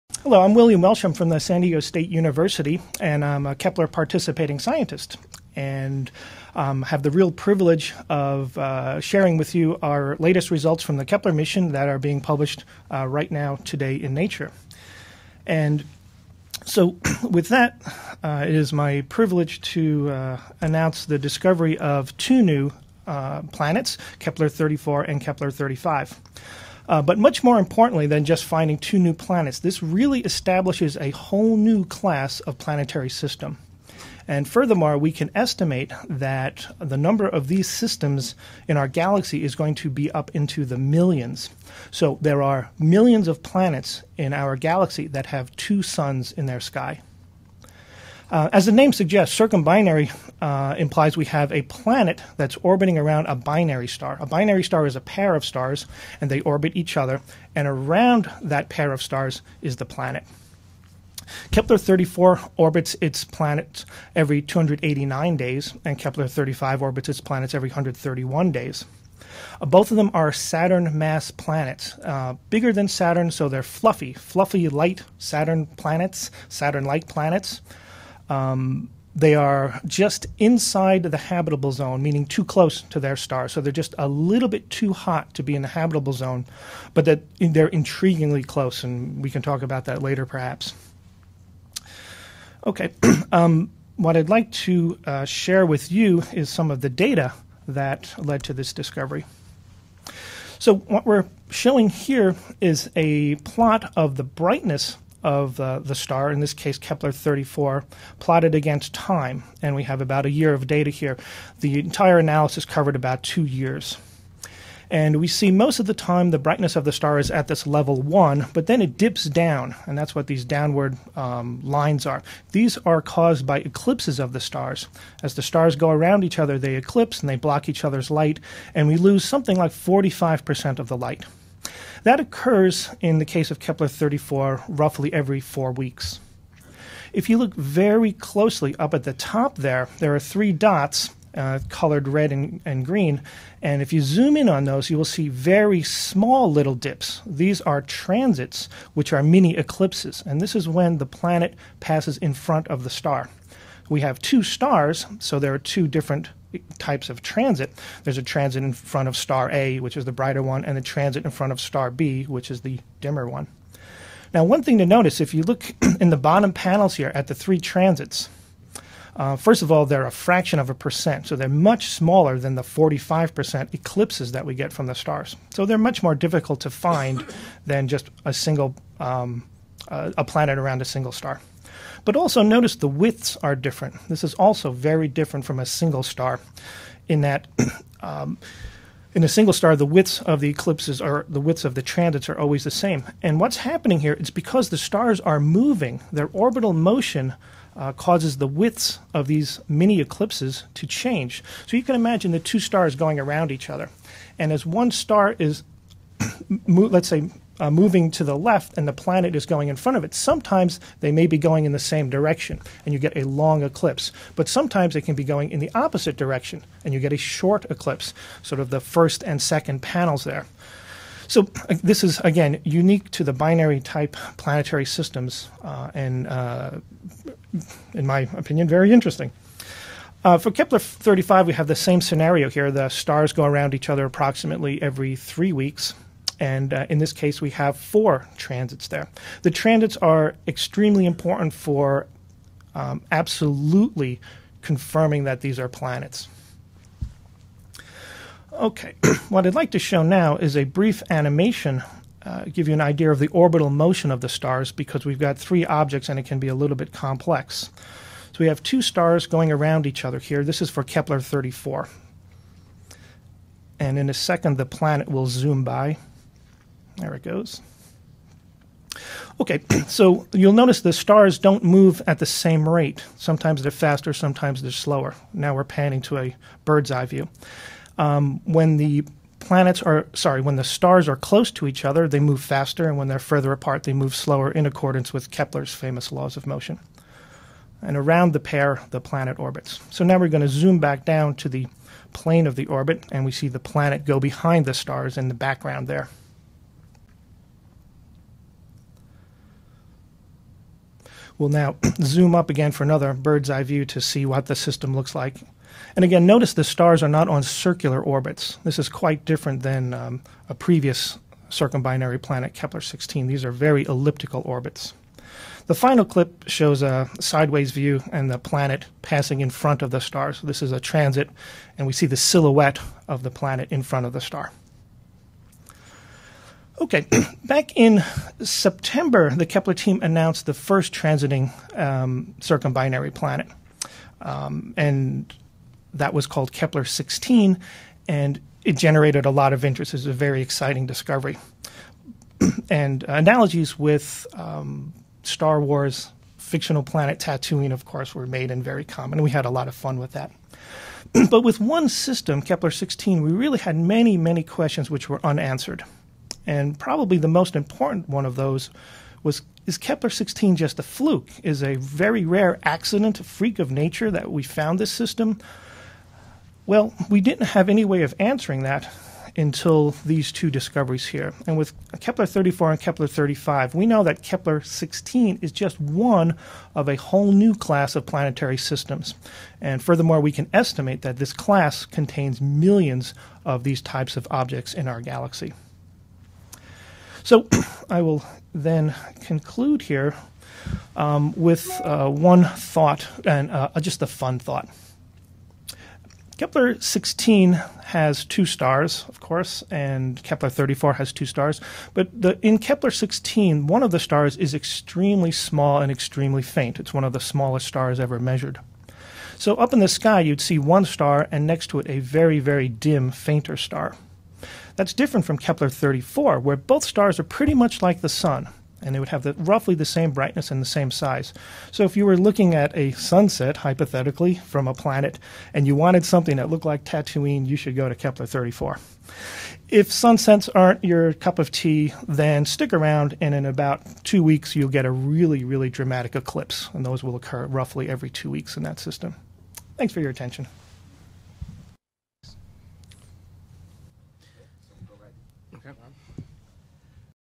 News About Kepler 34b and 35b
Latest news about Kepler's discovery of two more planets orbiting binary star systems.